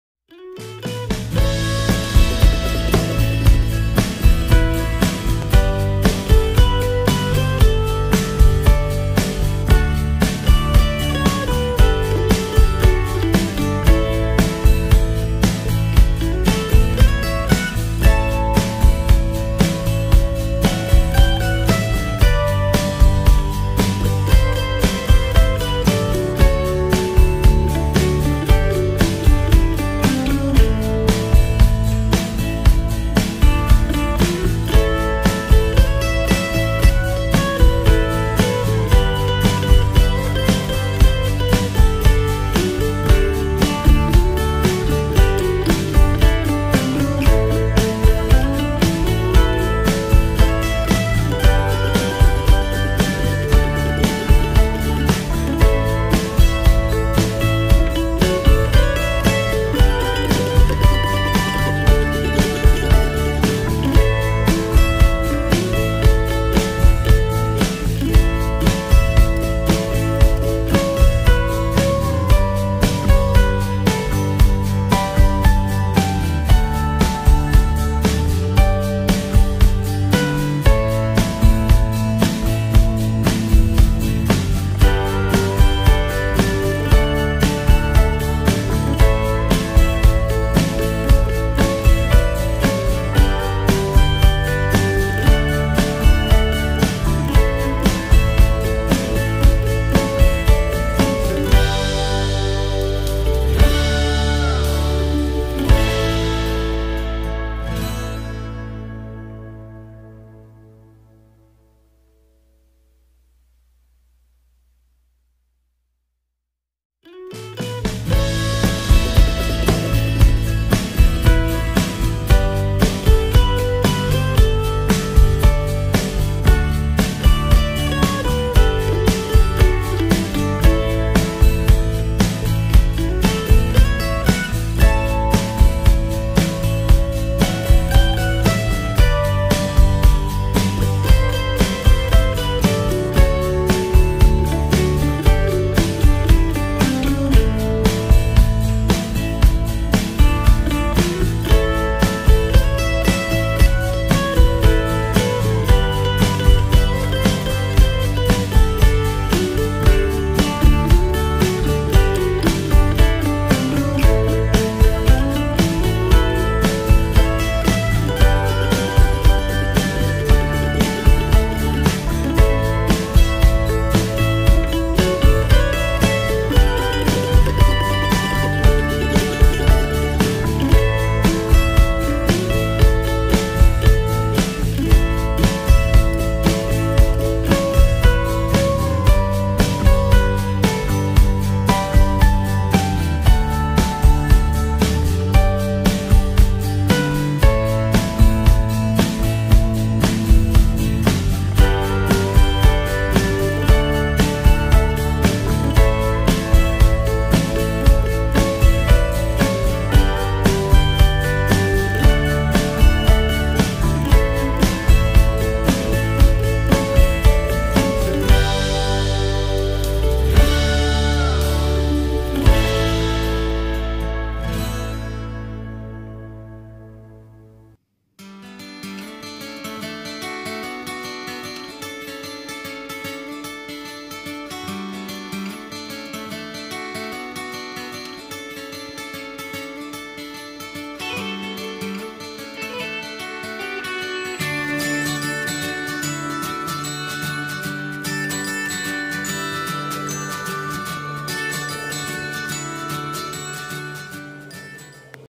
犬手作りケーキ【ごちになります】緑内障バセットハウンド犬と猫 Homemade cake.15years Old A Blind dog
去年はガブガブ食べていましたが、それでも半分は食べてくれたのでよかったです。
(二歳で両目緑内障、全盲義眼シリコンのバセットハウンド犬　)
( Binocular glaucoma at 2 years.Total blind eye silicone.Basset hound dog)

Two years old with both eyes glaucoma, occlusion silicone blind dog Basset Hound.
Cat that protected 2013.